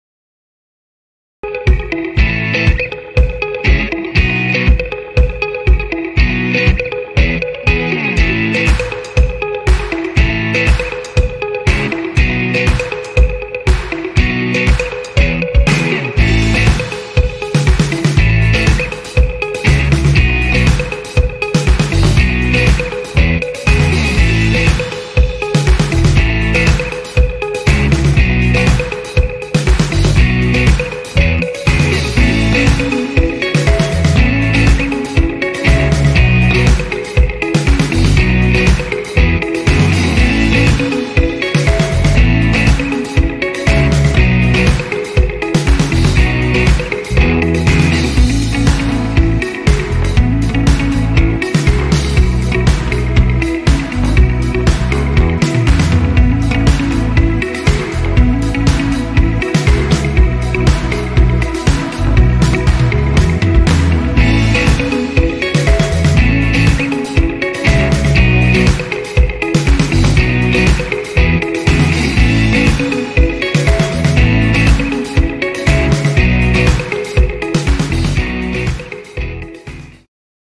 Where to Avail Cuevas Medek Exercises for Your Child in Dubai?
In order to make your child able for independent walking, avail the Cuevas Medek exercise therapy treatment from HOPE AMC.